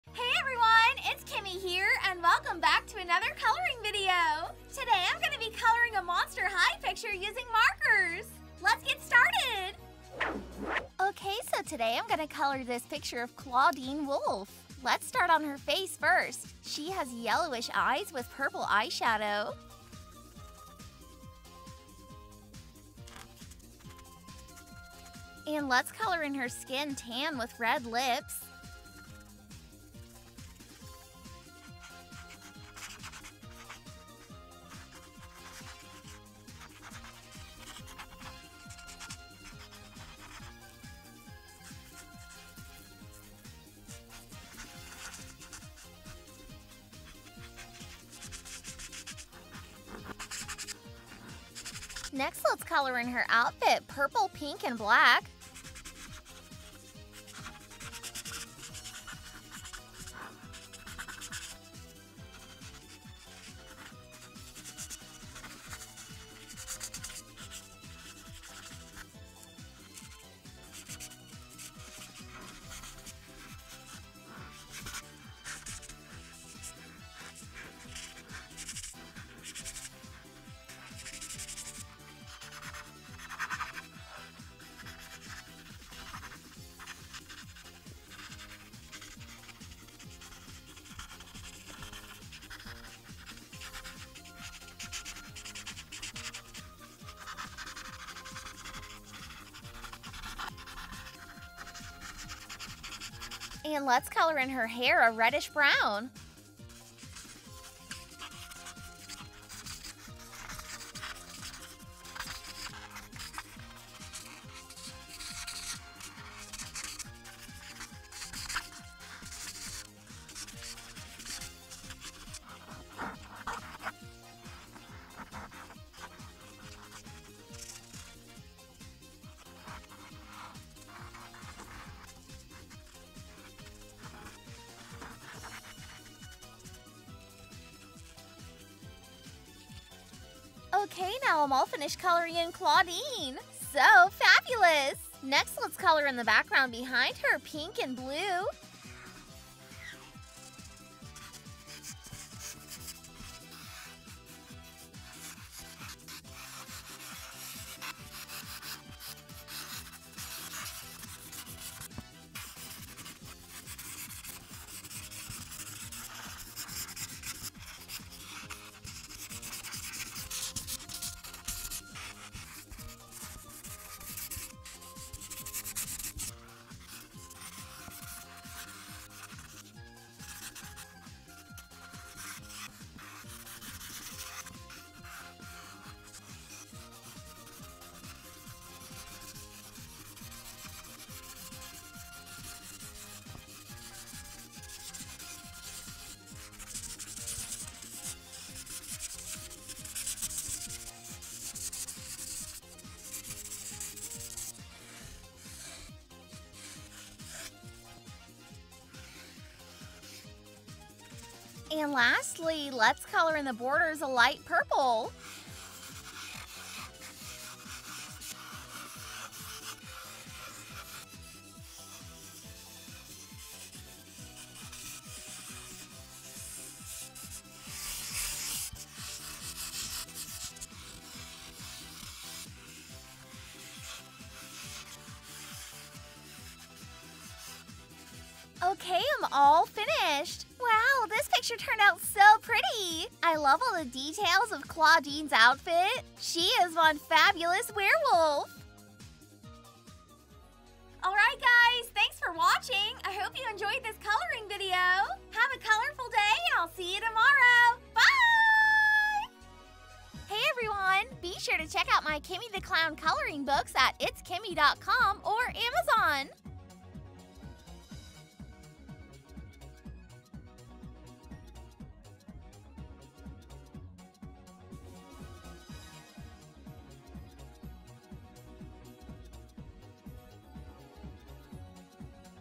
Monster High Clawdeen Wolf Coloring Page | Ohuhu Paint Markers
Hey everyone, and welcome to Coloring With Kimmi The Clown! Watch me color a picture of Clawdeen Wolf using Ohuhu colored paint markers! I had so much fun coloring in this awesome book and watching my picture come to life! Great book for people who love Monster High, coloring, drawing, painting or being artistic! HAVE A COLORFUL DAY!!! ☺️